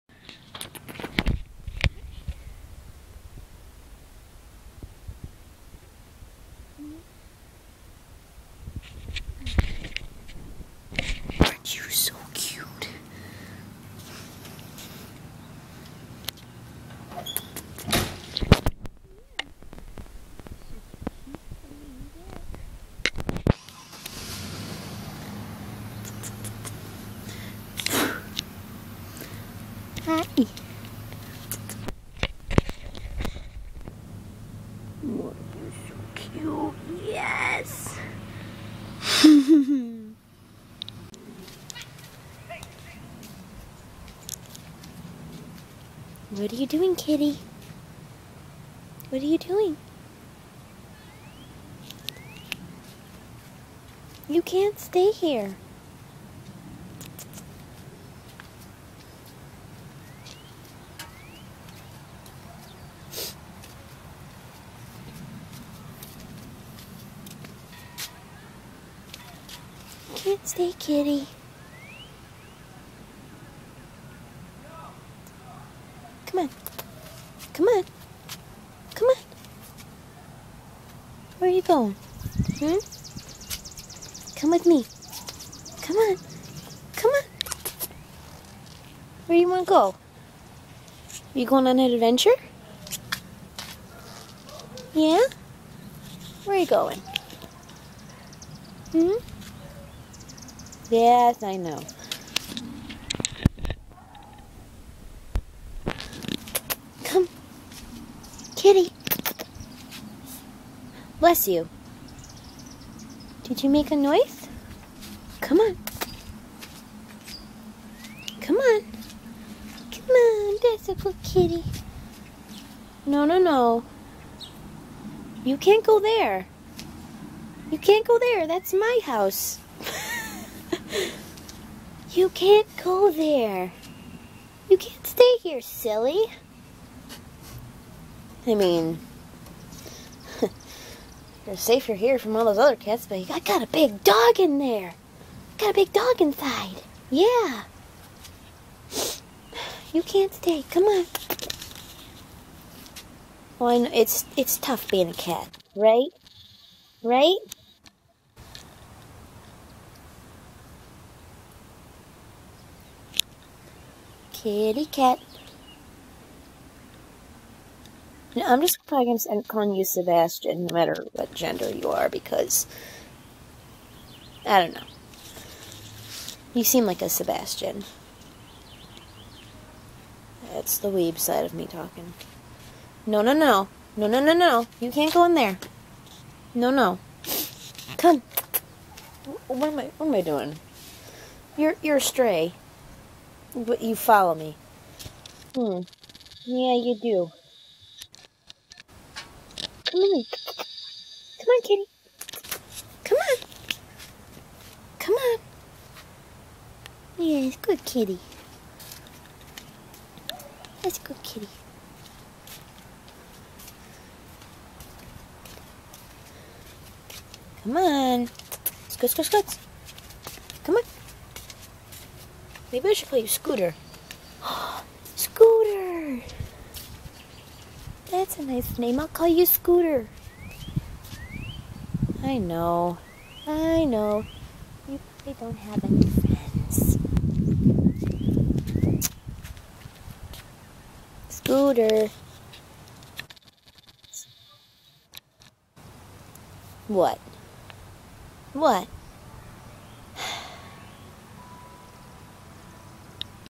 Playing with the neighborhood cat
Just one of the stray cats that wander our neighborhood.  :)  She's very sweet too~  (( Just found out the cat's sex after one of the neighbor's tried to take her in ))